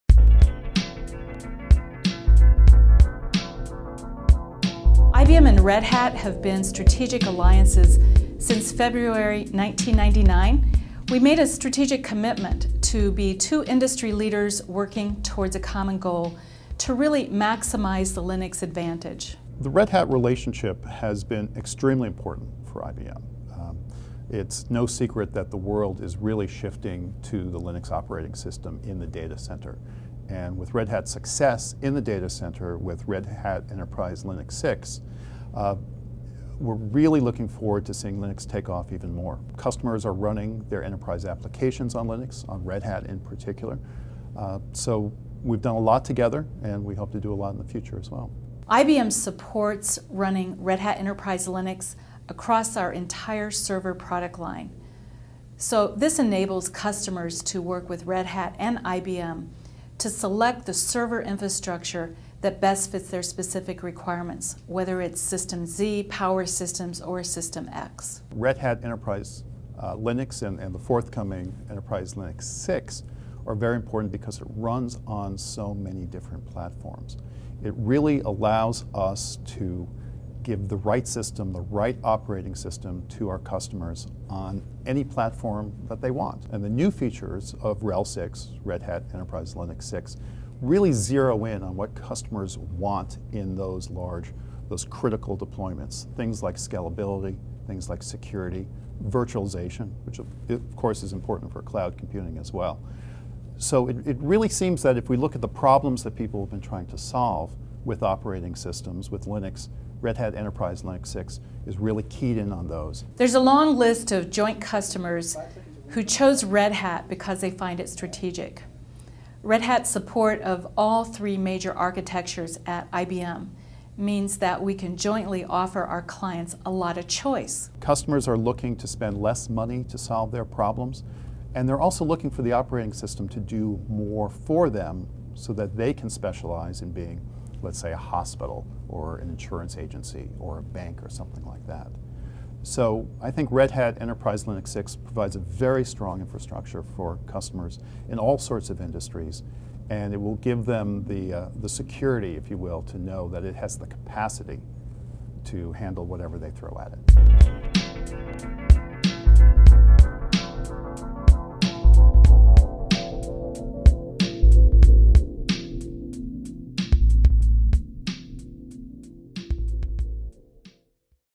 IBM relies on Red Hat
Jean Staten Healy, director of cross-IBM Linux, and Bob Sutor, vice president of open systems and Linux (both at IBM) discuss the long-time strategic alliance with Red Hat and the shift to Linux for enterprise computing in the technology industry. They discuss the flexibility of Linux across all IBM hardware architectures, and the continuing advancements in areas of virtualization and cloud computing, scalability, security, and reliability. They also reference continuing work and current projects in mission-critical spaces in the healthcare, telecommunications, and the datacenter in general.